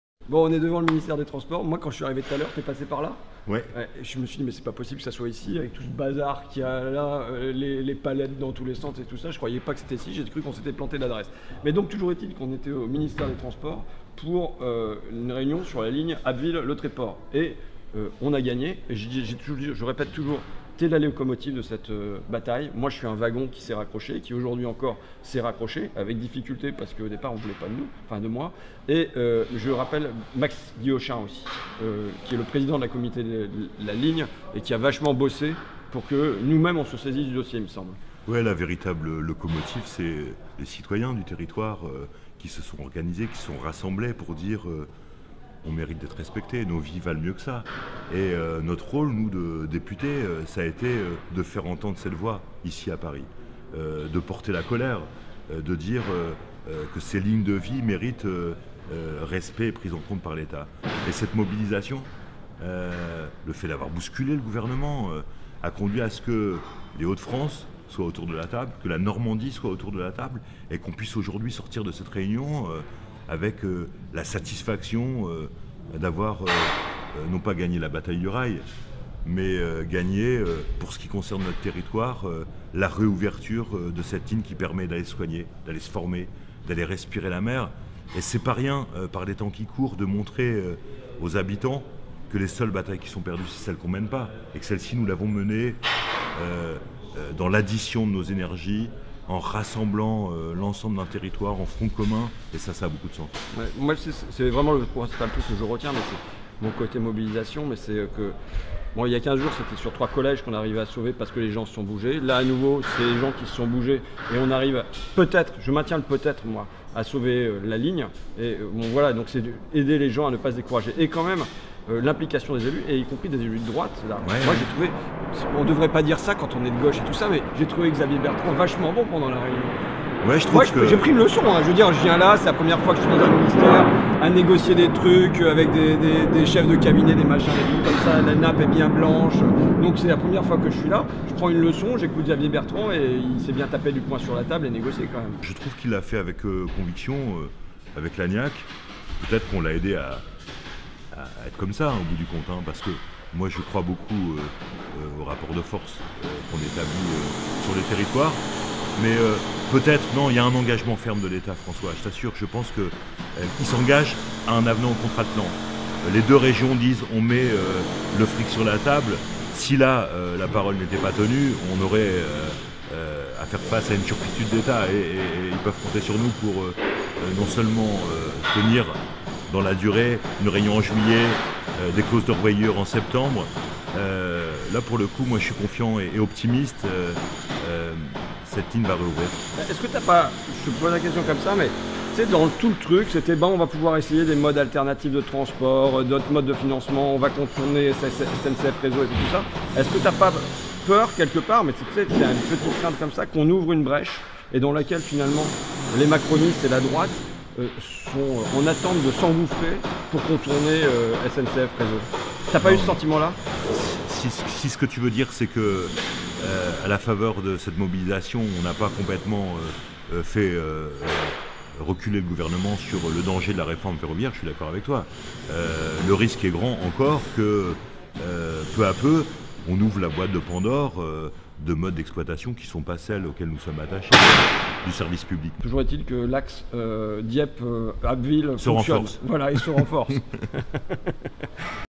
François Ruffin : Victoire pour la ligne Abbeville-Le Tréport !
Victoire pour la ligne Abbeville-Le Tréport, avec la mobilisation du camarade Sebastien Jumel, du collègue Emmanuel Maquet, de Xavier Bertrand, et surtout, surtout, des citoyens : la vraie locomotive, c'est vous !